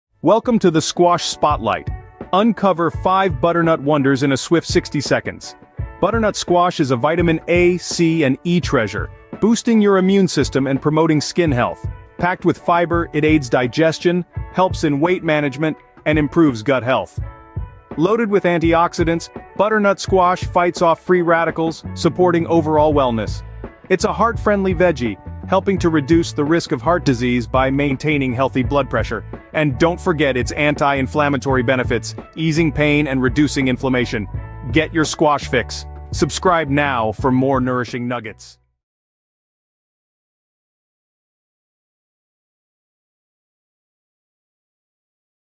Squash Spotlight: 5 Butternut Wonders Unveiled🍁🔥
🍁 Step into the world of Butternut Squash with our cozy guide! This autumn favorite 🍂 is not just delicious; it's a powerhouse of nutrients.

🌟 Rich in vitamins A, C, and E, butternut squash boosts your immune system and promotes glowing skin. Its high fiber content supports digestion, aids in weight management, and improves gut health. 🌿

🔥 Loaded with antioxidants, it combats free radicals and enhances overall wellness. It's also heart-healthy, helping to maintain blood pressure and reduce heart disease risks. ❤️

💪 Experience its anti-inflammatory benefits, perfect for reducing pain and swelling.

🎥 Watch our video to unveil butternut squash's nutritious secrets!